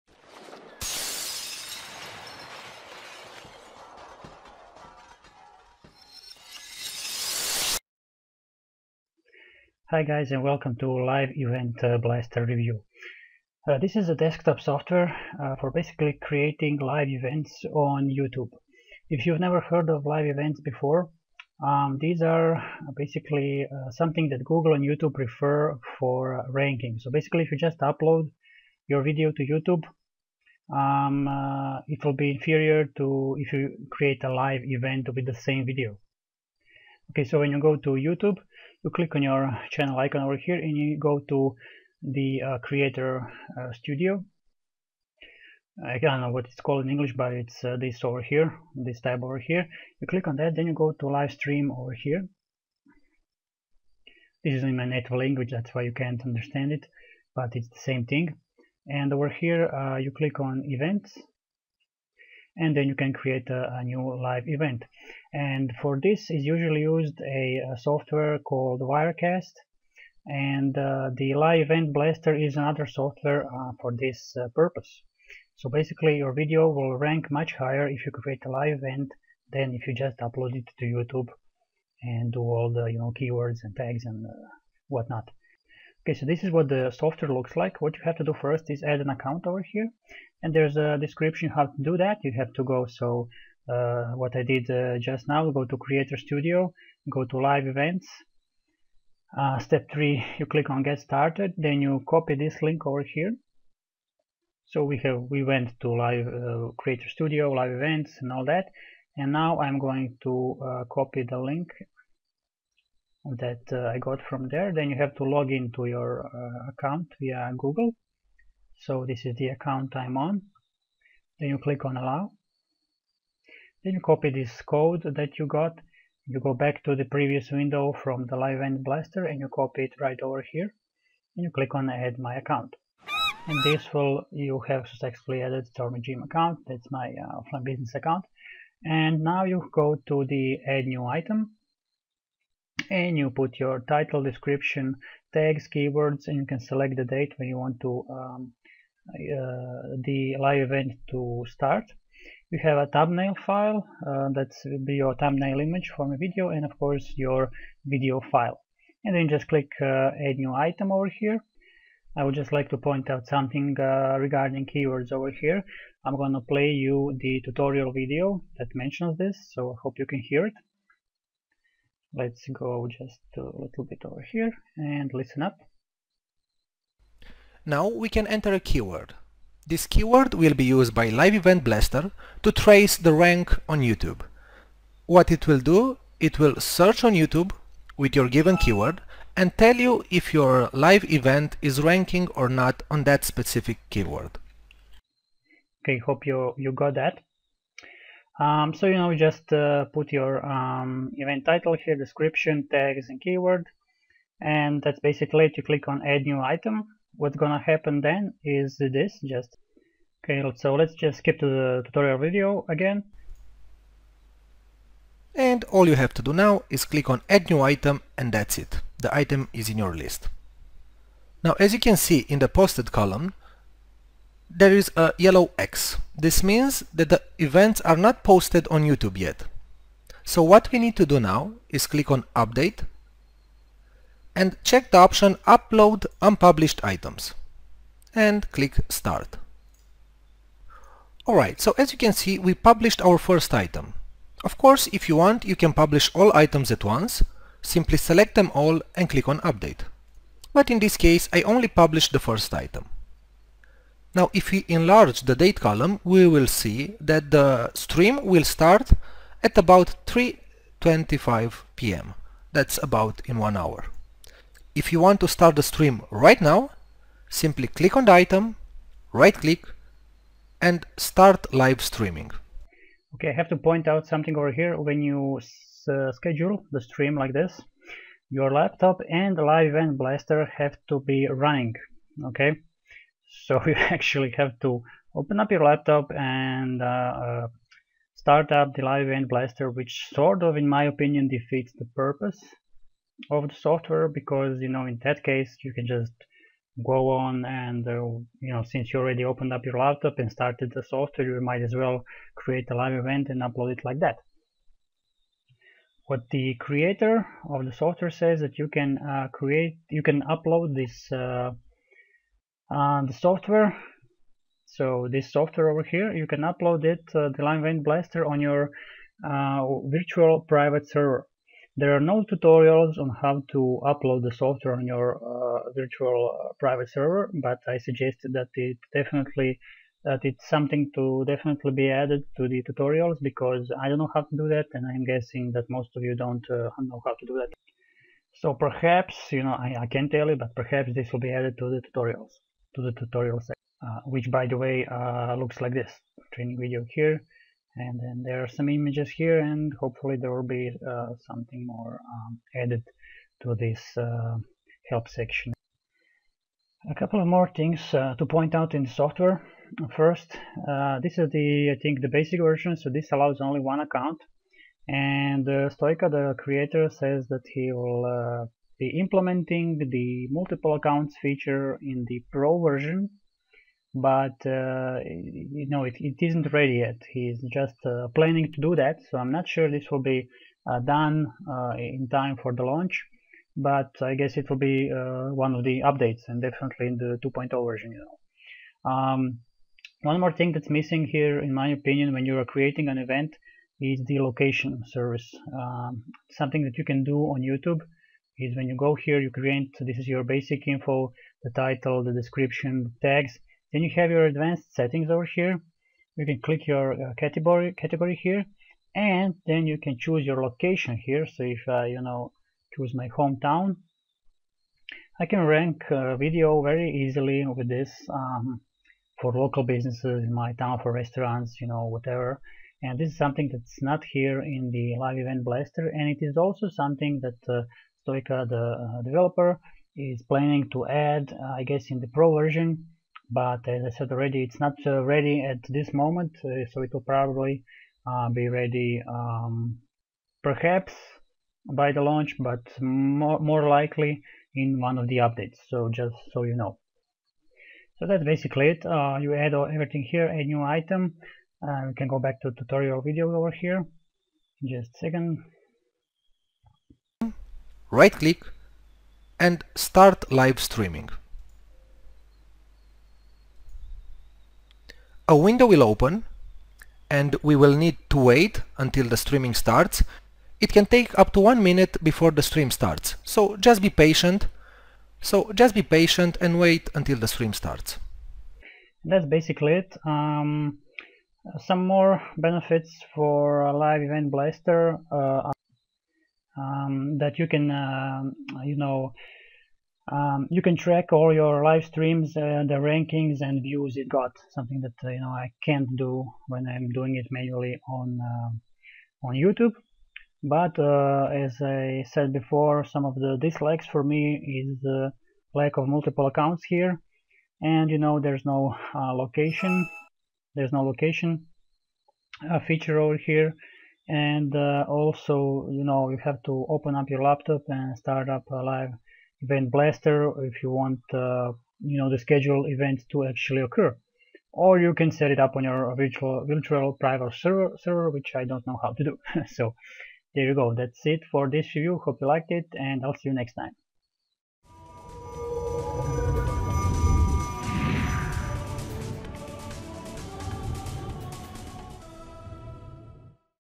LIVE EVENT BLASTER HONEST REVIEW
Master Blaster

Ok I basically say everything in the video but here it is in short :)

What is Live Event Blaster? If you never heard of live events before. These are something that Youtube and Google of course prefer for ranking your vidoes.

So Live Events have an advantage over regular uploads. And no that doesn't mean you have to be live in the camera, in da face, shooting yourself as you share the wisdom of Solomon... I tell you these things just come out as I go :p You can just upload a recorder video file.

So what da heck is it and what does this Live Event Blaster actually do do do?

It's a desktop software for creating and scheduling live events. Usually a software called Wirecast is used to do this, well this is an alternative.